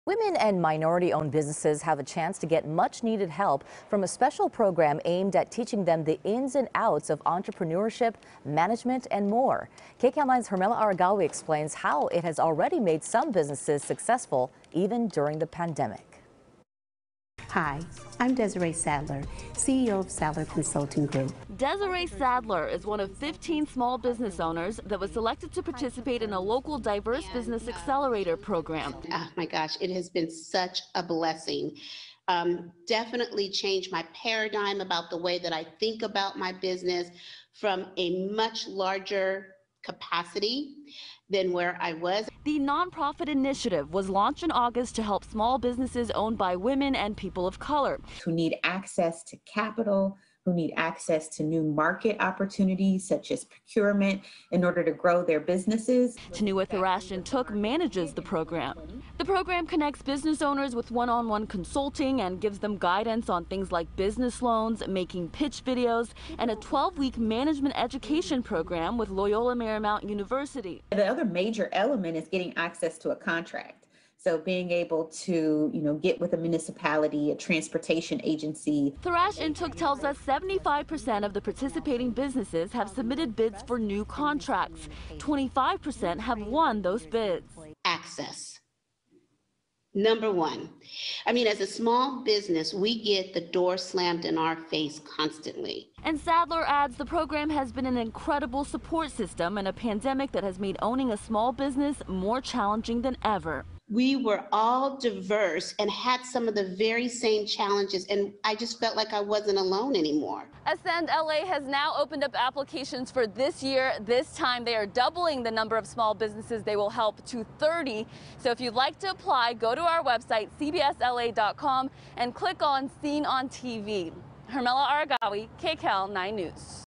Los Angeles Nonprofit Aims To Uplift Businesses Owned By Women, People Of Color
Ascend Los Angeles, a non-profit initiative, was launched in August to help small businesses owned by women and people of color.